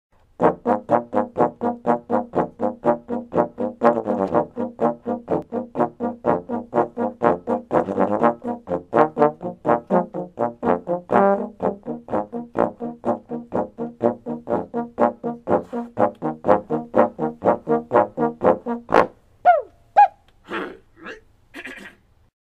luggage music for three bones
picking up my luggage in montreal

recorded in my basement with a tripod held together by tape